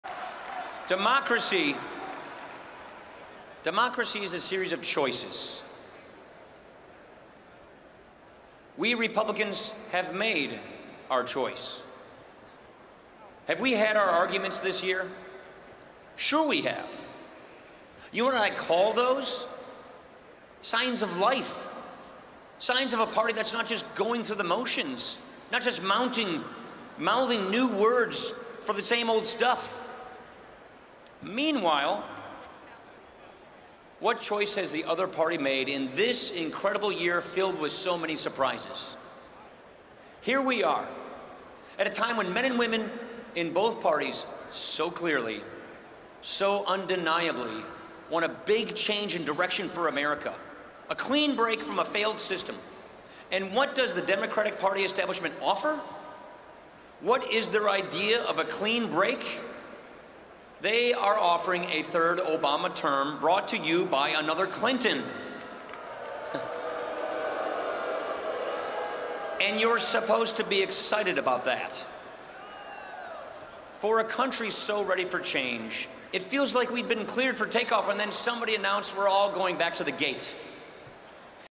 Paul Ryan: 'Democracy is a series of choices'
House Speaker Paul Ryan outlined the differences between the Republican and Democratic parties, saying that any fights among the GOP were "signs of life" and a path to change. Ryan spoke at the 2016 Republican National Convention in Cleveland, Ohio on Tuesday.